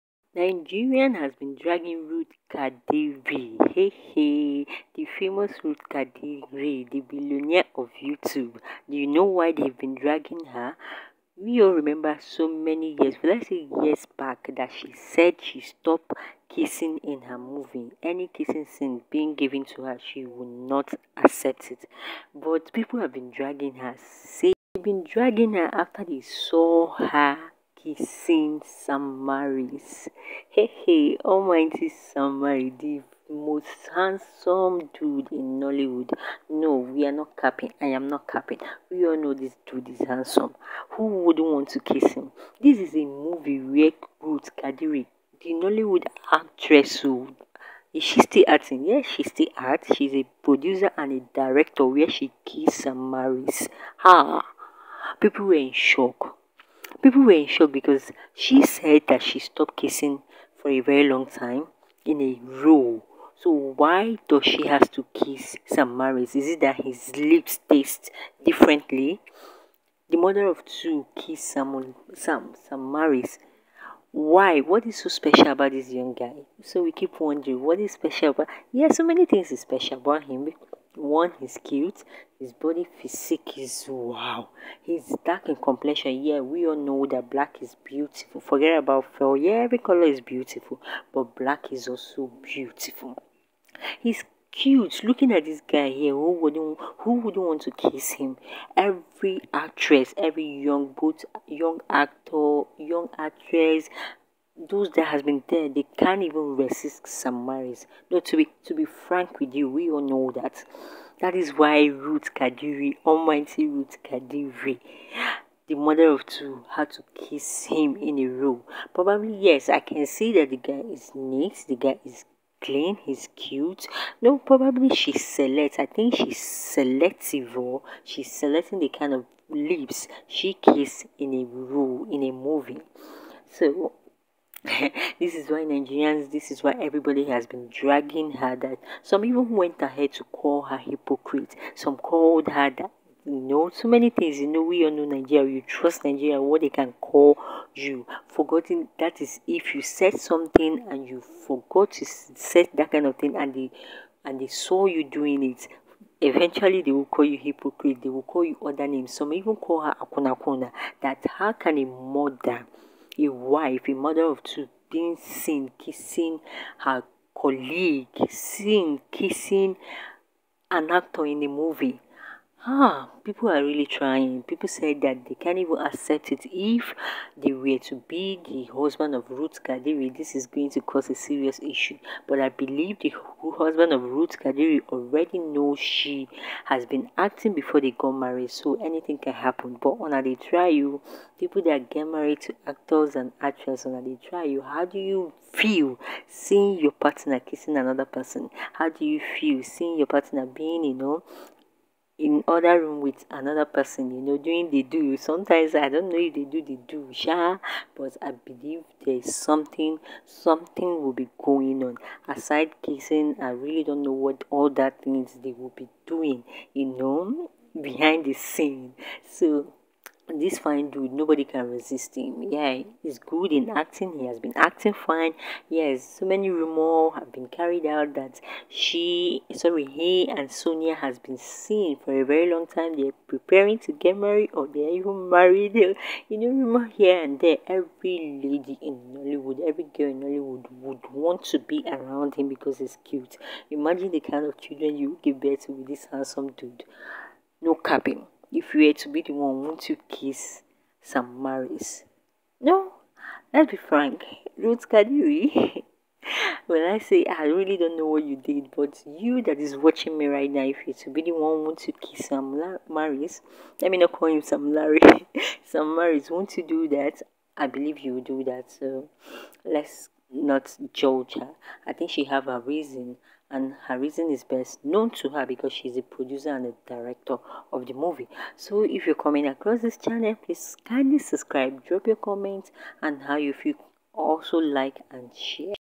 WHY I KISSED SAM MAURICE ON A SET. RUTH KADIRI
Ruth kadiri explain why she kissêd Sam Maurice on a movie set.